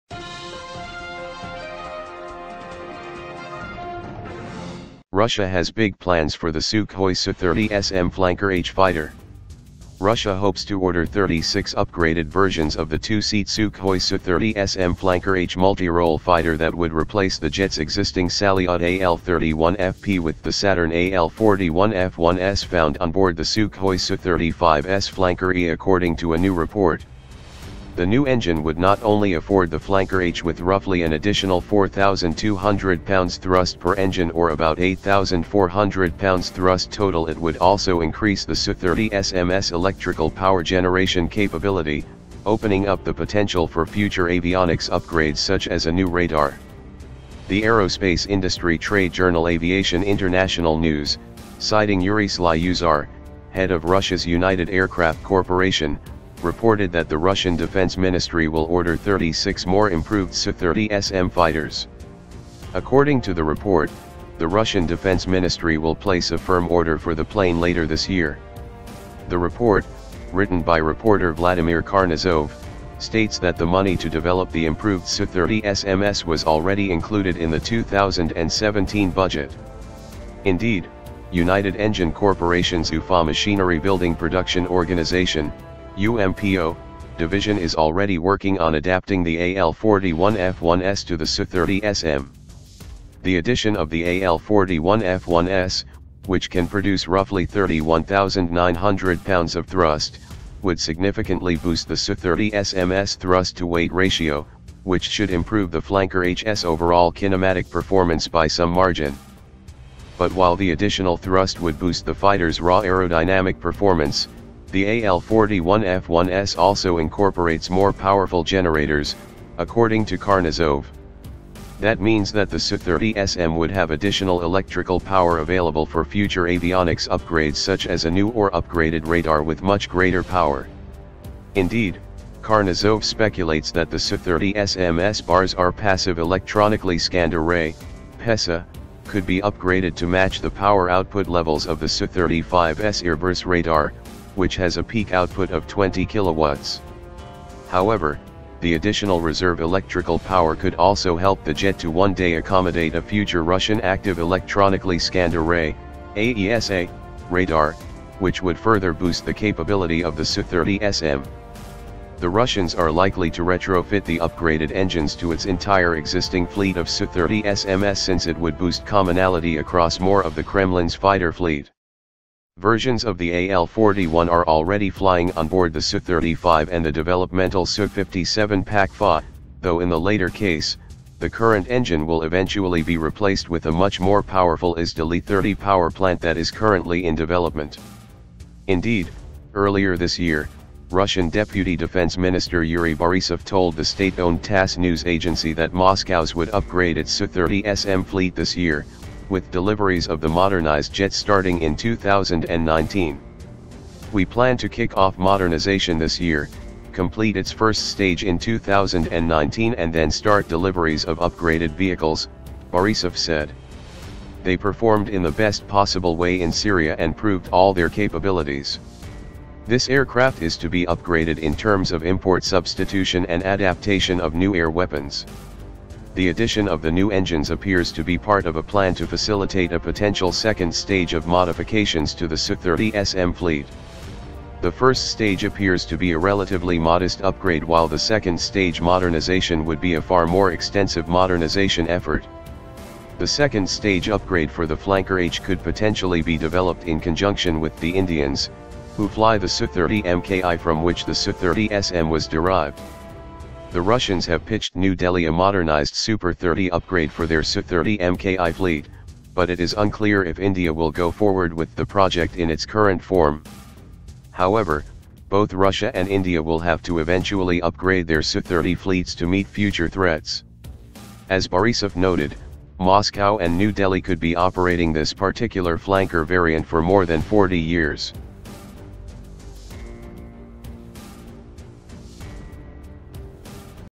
Russia Has Big Plans for the Sukhoi Su-30SM Flanker-H Fighter
Russia hopes to order 36 upgraded versions of the two-seat Sukhoi Su-30SM Flanker-H multirole fighter that would replace the jet’s existing Salyut AL-31FP with the Saturn AL-41F-1S found onboard the Sukhoi Su-35S Flanker-E according to a new report. The new engine would not only afford the Flanker-H with roughly an additional 4,200lbs thrust per engine—or about 8,400lbs thrust total—it would also increase the Su-30SM’s electrical power-generation capability, opening up the potential for future avionics upgrades such as a new radar.

The aerospace industry trade journal Aviation International News, citing Yuri Slyusar, head of Russia’s United Aircraft Corporation, reported that the Russian defense ministry will order 36 more improved Su-30SM fighters. According to the report, the Russian defense ministry will place a firm order for the plane later this year. The report, written by reporter Vladimir Karnozov, states that the money to develop the improved Su-30SMs was already included in the 2017 budget. Indeed, United Engine Corporation’s Ufa Machinery-Building Production Organization (UMPO) division is already working on adapting the AL-41F1S to the Su-30SM.